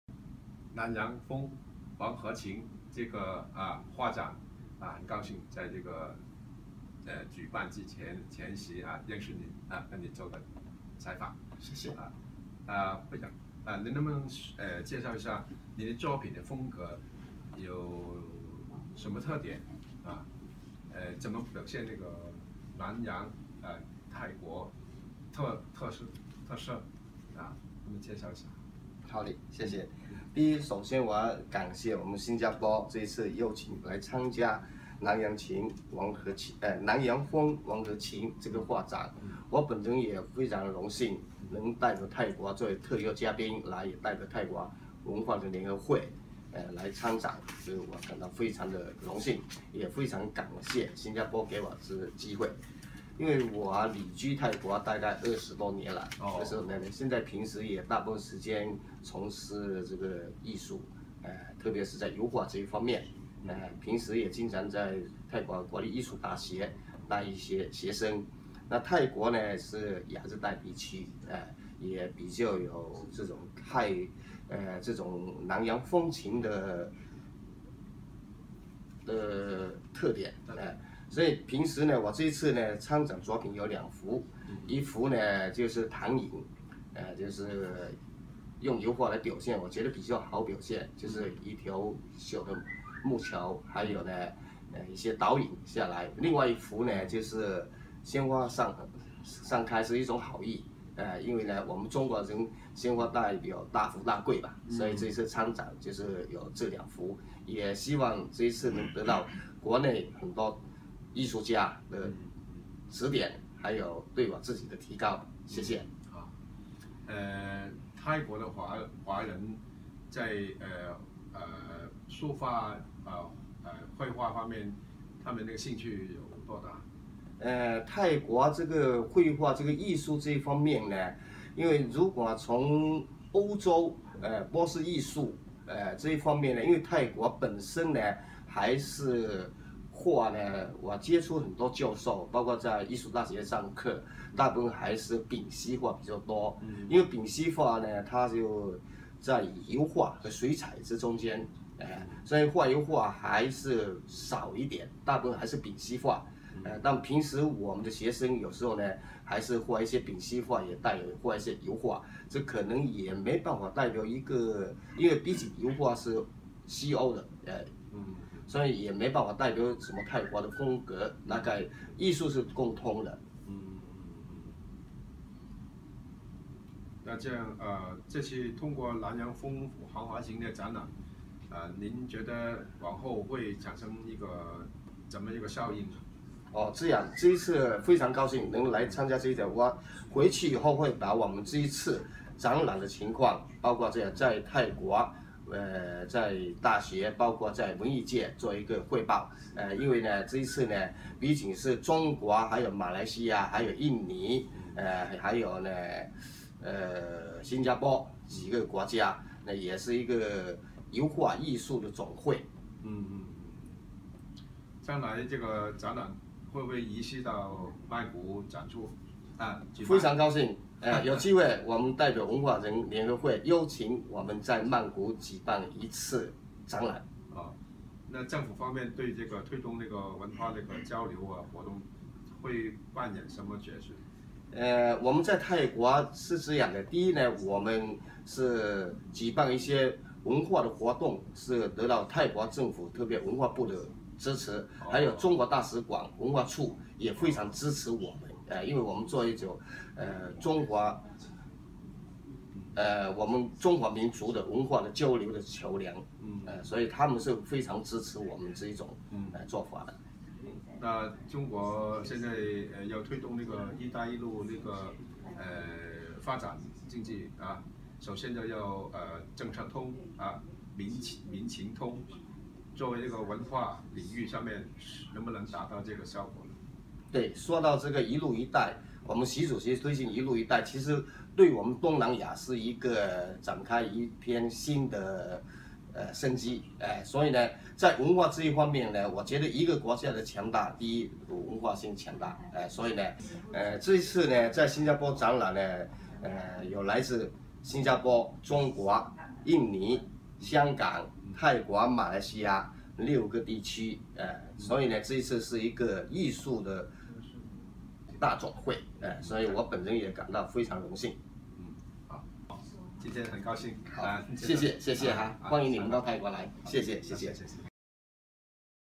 2016-1-31“南洋风•黄河情”油画家写生艺术大展 – 吳令平
“南洋风•黄河情”油画家写生艺术大展

今年开年来新加坡画坛传来重大喜讯，荟集了新加坡、中国、马来西亚、印尼、泰国与香港著名油画家于一炉的【“南洋风•黄河情”新加坡、中国、马来西亚油画家写生艺术大展】，在2016年1月31日下午2时30分，由新加坡文化、社区及青年部财政部高级政务部长沈颖莅临主持下拉开序幕。

为了加强本展的阵容，新加坡另外特邀本地96岁的画坛泰斗林子平，以及前新闻及艺术部高级政务次长、新加坡艺术联合总会会长何家良加盟此次展出。

除新马之外，来自印尼的东盟•中国书画家联合总会总会长阮渊椿、泰国泰中文化人联合会会长吴令平、香港锺建新艺术馆馆长锺建新也在本展特邀之列，以便更全面地体现南洋风及反映本区域的油画艺术水平。

新加坡美术总会会长梁振康强调，这是新、中、马三国油画家第一次集体受邀到河南进行写生，大家在亲身领略了黄河文化以及风土人情后，分别用自己的画笔精心描绘的油画写生创作，这批作品不仅反映今日的黄河风貌，也更能让观展者从中感受到黄河那不可抵挡的魅力。

本展组委会总顾问陳光億大师希望，这两个分别具有鲜明地域特色的优秀油画作品，所带来的冲击效果，能够促进彼此之间的了解，加强海内外油画家的凝聚力，在让世人了解黄河，了解南洋两个地域风情的同时，也关注我们东方油画艺术在开拓之路上所取得的成就。

NANYANG LINK WITH HUANG-HE SPIRIT ART EXHIBITION

We will be celebrating a great event at the Singapore beginning of the year, through assembling celebrated artists from China, Hong Kong, Indonesia, Malaysia, Thailand, and Singapore. This exhibition event will be officially opened by Ms Sim Ann, Senior Minister of State, Ministry of Culture Community & Youth, and Ministry of Finance on 2016 January 31st, at 2:30PM.

This exhibition is an essential cultural exchange between China and the ASEAN countries. There will be 2 series of art works on display –Nanyang Link, and Huang-He Sprint contributed by the ASEAN artists together with China’s prominent artists. The landscape paintings of Huang-He Sprint were drawn at the China Sanmenxia Yellow River site in March 2015. The Huang-He Sprint was created by painters Leong Chun Hong Stephen and Dr. Foo Yong Kang from Singapore, five Malaysian oil painters, and Mr. Fan Di Ann, President of China Central Academy of Fine Arts, led five painters from China.

Works of Singaporean artists Leong Chun Hong, Stephen, the President of the Federation of Art Societies (Singapore), and other local veteran oil painters Koeh Sia Yong, Fan Shao Hua, Lim Poh Teck, Ler Hock Chuan will also be expected to draw attention at the exhibition.

We have also invited two of the most renowned local artists, Mr. Lim Tze Peng, and Dr. Ho Kah Leong to take part in this multinational painting exhibition. Dr. Ho Kah Leong is the President of Singapore Arts Federation, Ministry of Information and the Arts and former Senior Parliamentary Secretary. Mr. Lim is the 2003 Cultural Medallion Award winner.

The Chief Adviser of the Preparatory Committee, Master Janson Tan, heartily hopes that the cultural impact of combining the two distinctive regional paintings characteristics will not only promote mutual understanding, but also strengthen the network of painters worldwide. This is a fantastic opportunity for the world to appreciate the customs of the China Yellow River Basin and the ASEAN countries (Nanyang). It is worth understanding how they have explored and developed the art of oriental paintings that we see in today’s achievements.